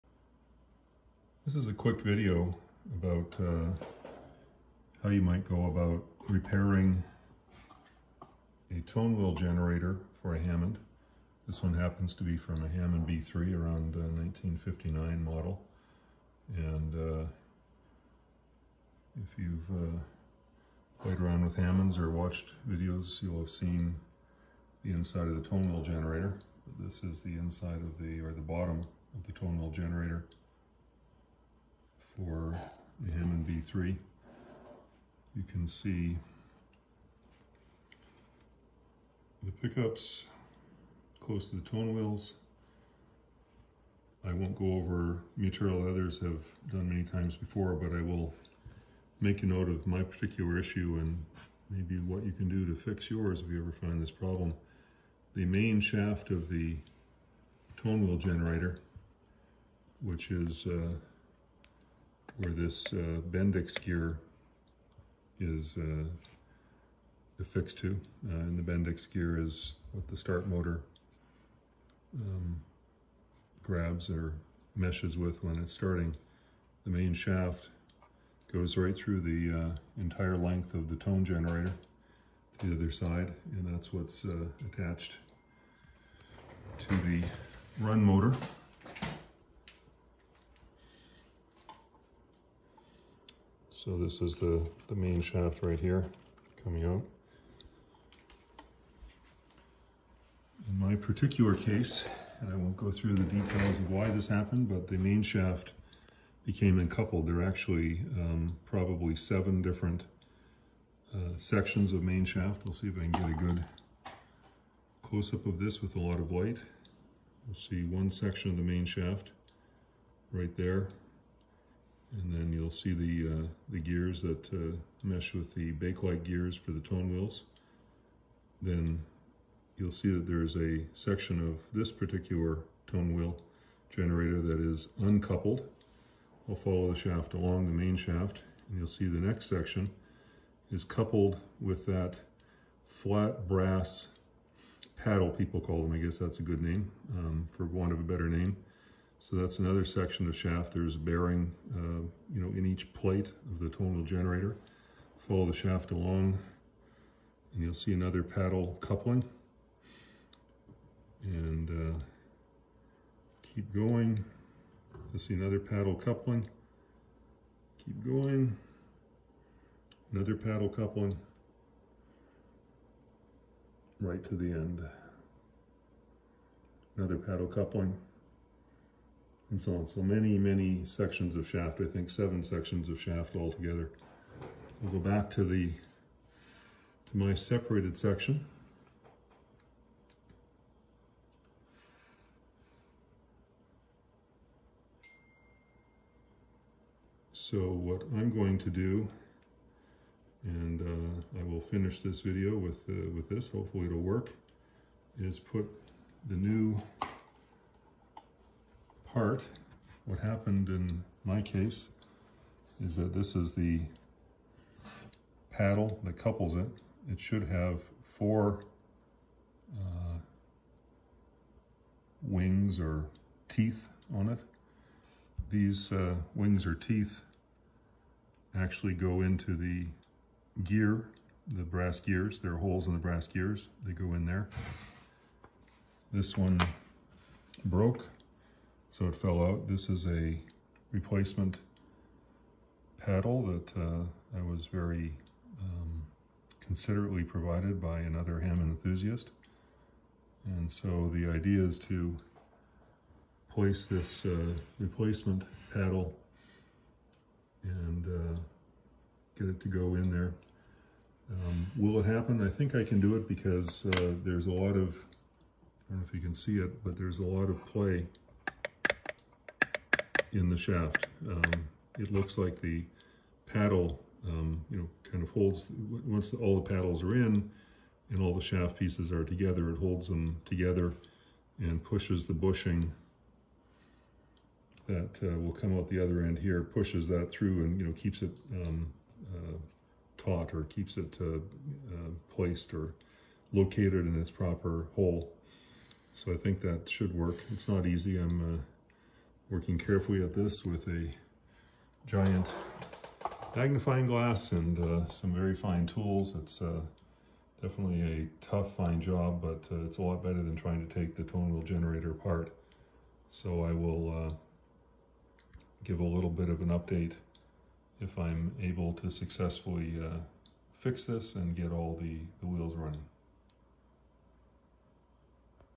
Repairing an “uncoupled” Hammond Tone Wheel Generator shaft Part 1
The tone wheel generator from my 1959 Hammond B3 suffered a broken coupling or “paddle” which rendered the last part of the shaft useless, caused the start motor function to cease and cut off two bins of tone wheels. To repair it, I had to remove the tone wheel generator from the organ and fit a replacement paddle between the shaft sections. This is not a project for the faint of heart! This is part one because it was three or four hours of trial and error and frustration before I could figure out a method that worked. Part two reveals that method!